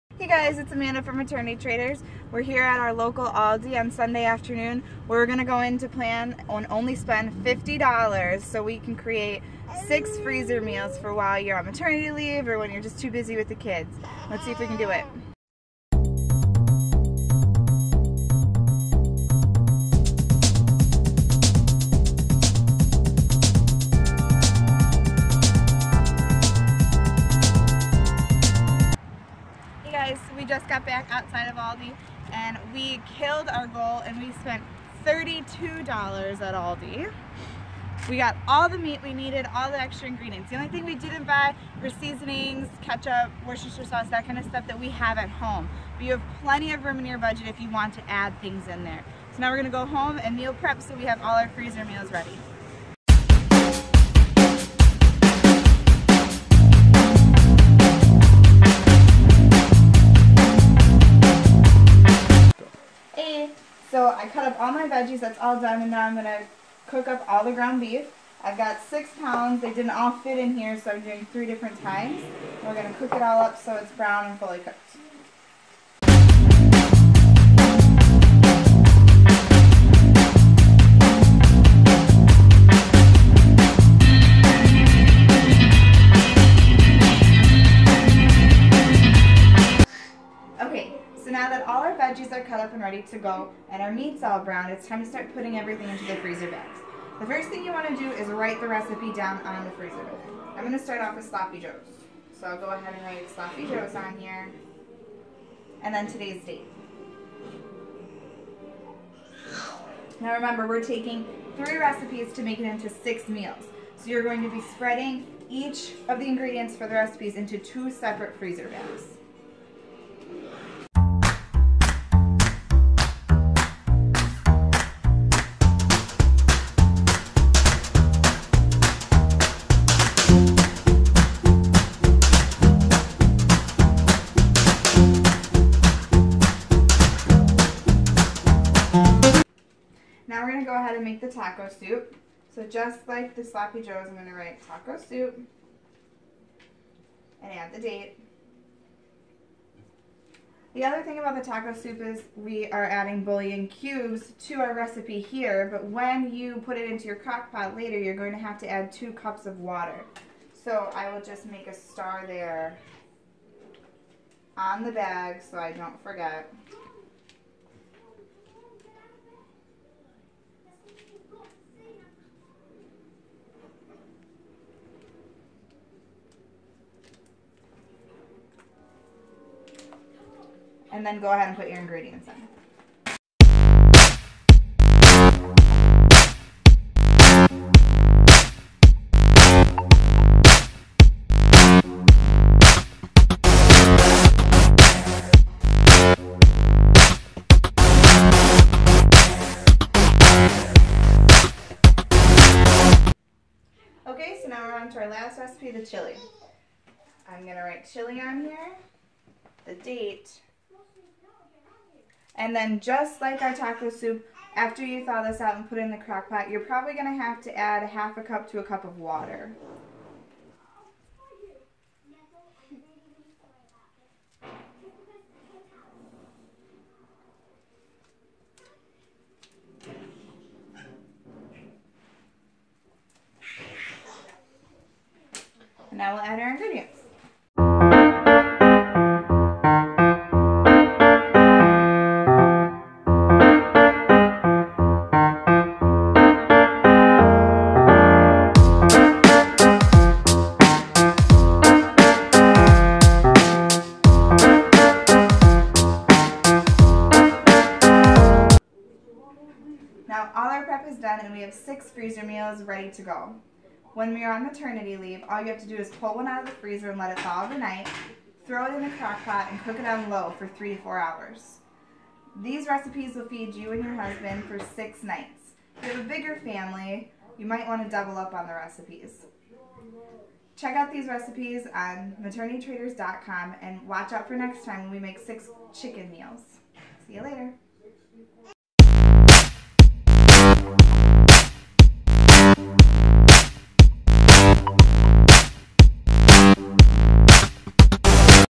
CHEAP Crockpot Meals for Maternity Leave
Here's a cool idea to prepare for maternity leave! Make a bunch of freezer crockpot meals before the baby comes and then dinner will be one less thing to worry about! PLUS don't blow the budget! Go to Maternity Traders to read the blog!